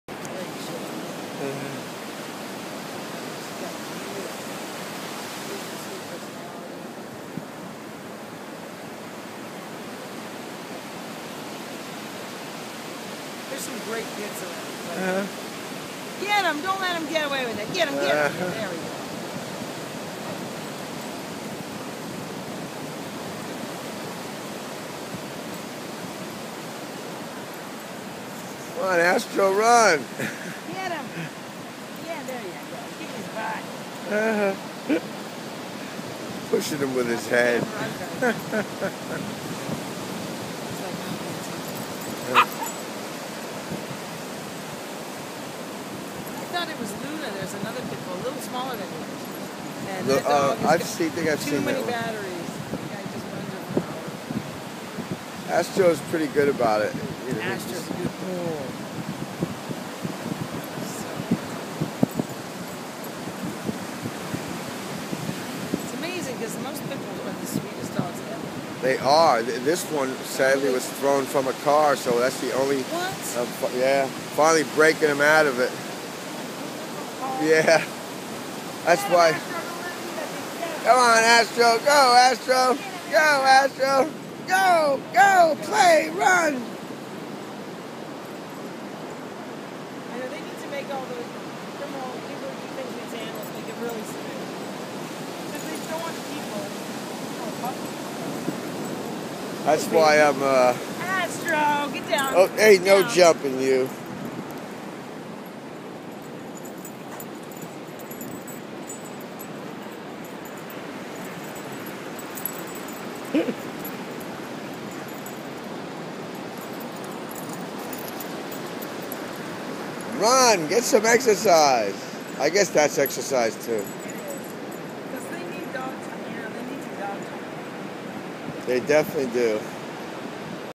ASTRO DOG PLAYING
ASTRO DOG PLAYING at the beach..my new rescue..